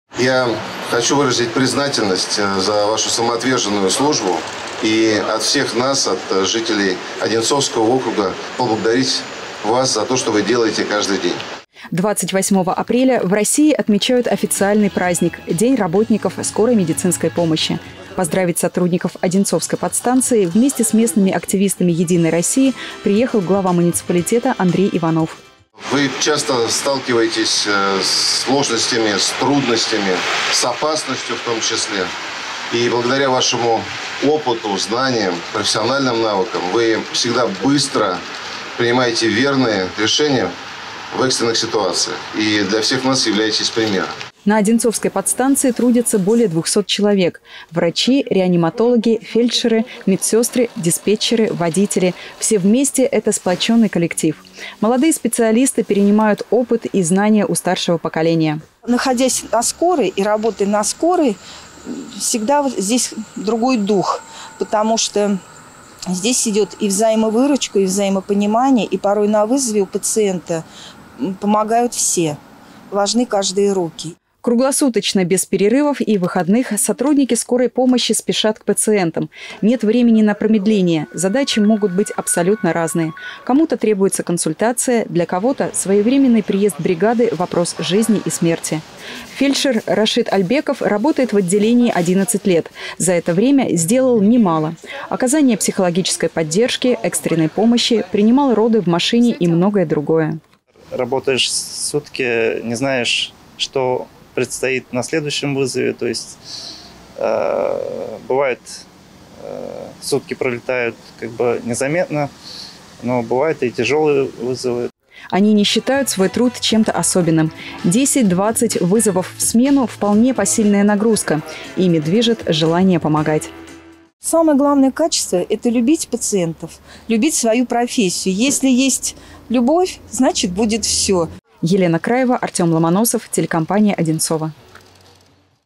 Глава поздравил сотрудников скорой помощи
Сотрудников Одинцовской подстанции скорой помощи с профессиональным праздником поздравил глава округа Андрей Иванов. Эти люди всегда находятся на передовой, их труд сложно переоценить. Они несут особую миссию, ежедневно оказывая экстренную помощь всем тем, кто в ней нуждается, и спасают человеческие жизни.